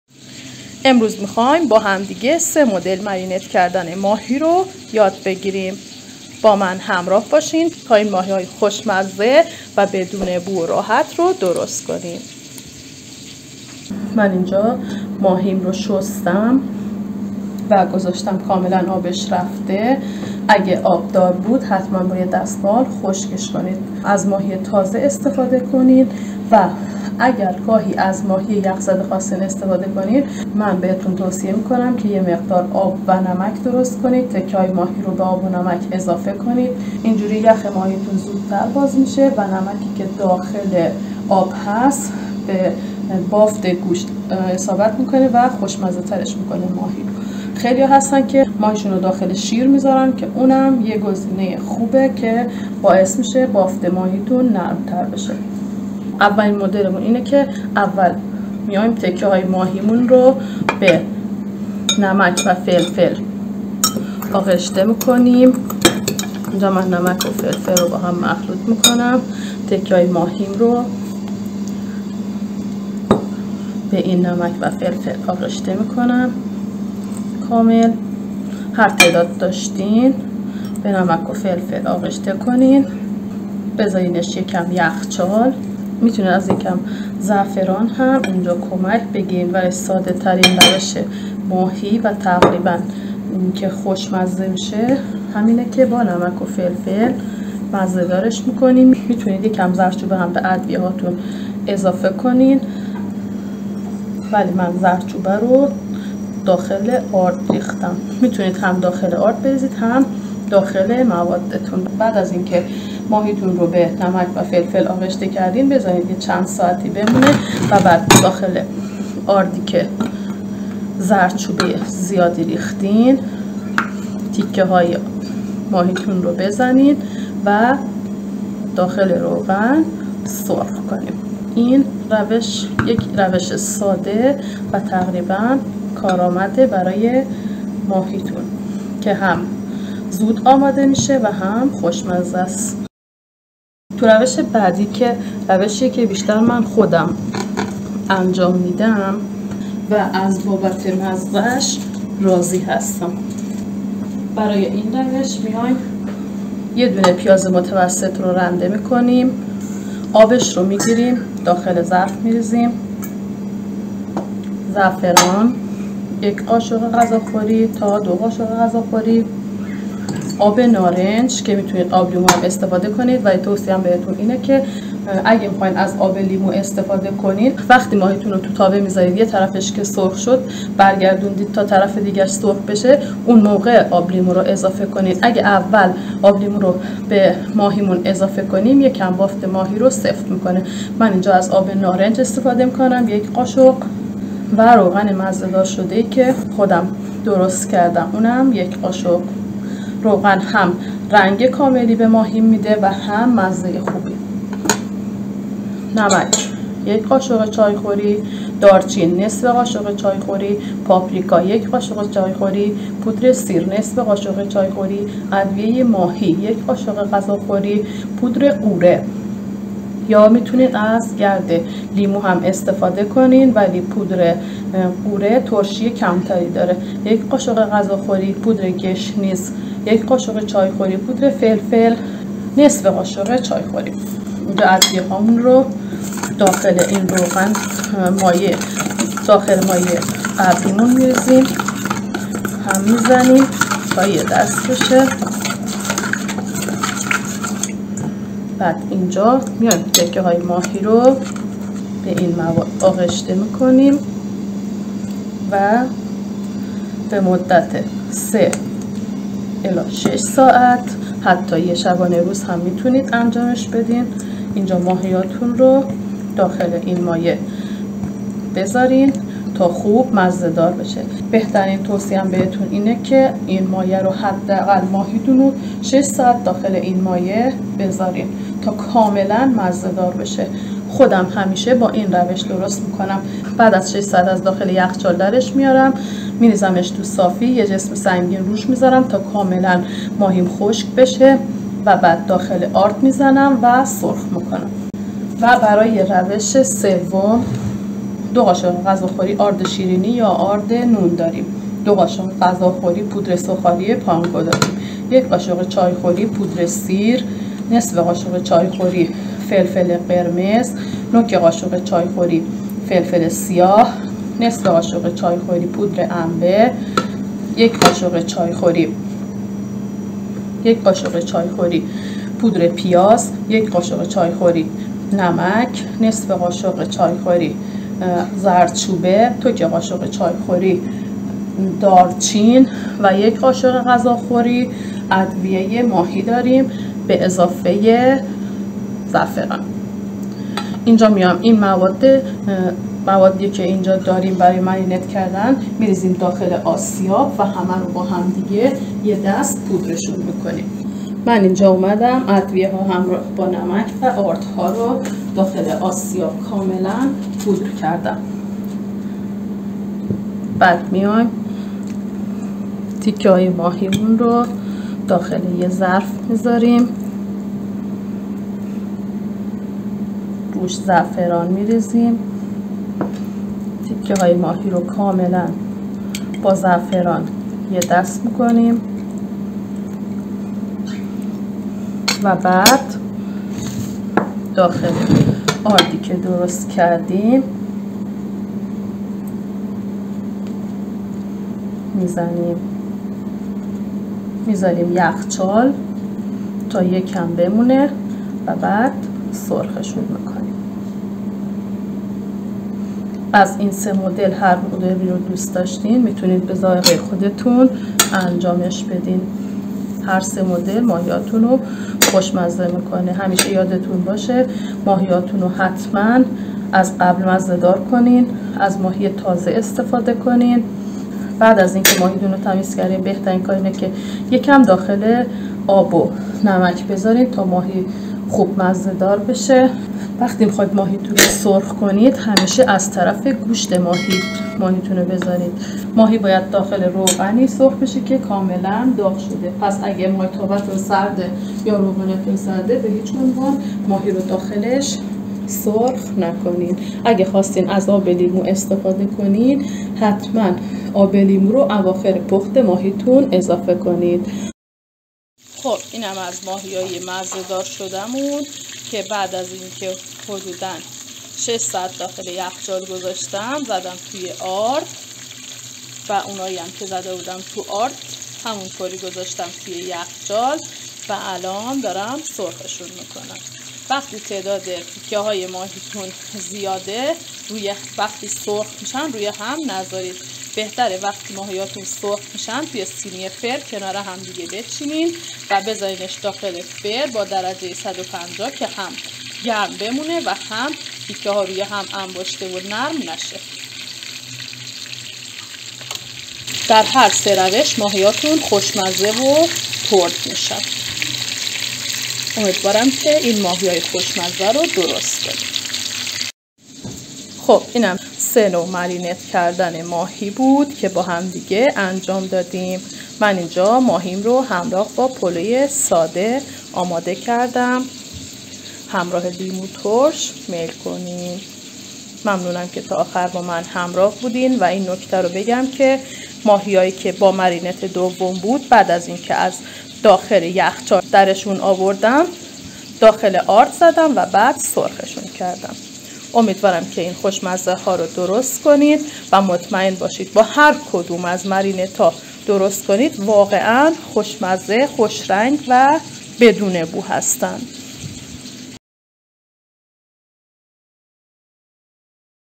بهترین روش های مزه دار کردن ماهی که هیچ بویی نمیده و فوق العاده ترد و خوشمزه میشه
سلام بچه ها امروز میخام بهتون بهترین روش های مزه دار کردن ماهی رو یاد بدم که ماهی تون دیگه هیچ بویی نمیده و کاملا ترد و خوشمره میشه پس این اموزش بی نظیر رو از دست ندین

00:00 توضیحات
00:14 نکات انتخاب ماهی مناسب
00:56 روش اول مزه دار کردن ماهی
02:24 روش دوم مرینت کردن ماهی
04:56 روش سوم مزه دار کردن ماهی و گرفتن بوی بد ماهی
07:35 سرخ کردن ماهی در ماهیتابه
08:27 سرو ماهی و تزیین ماهی